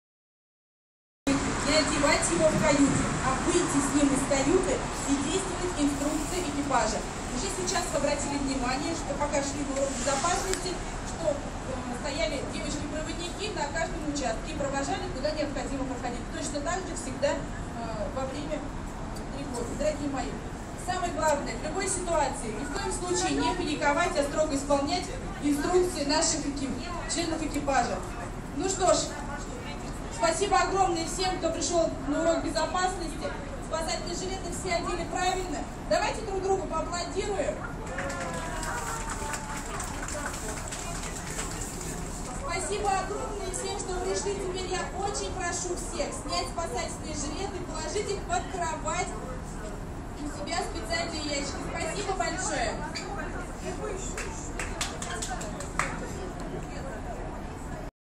Урок безопасности
Закрытие навигации на теплоходе "Александр Бенуа" компании Инфофлот в круизе по маршруту Москва – Углич – Ярославль – Городец (Трансфер в Нижний Новгород) («Закрытие навигации») с 7 по 10 ноября 2014 года.

 Все туристы со своими спасательными жилетами собираются в концертном зале на шлюпочной палубе.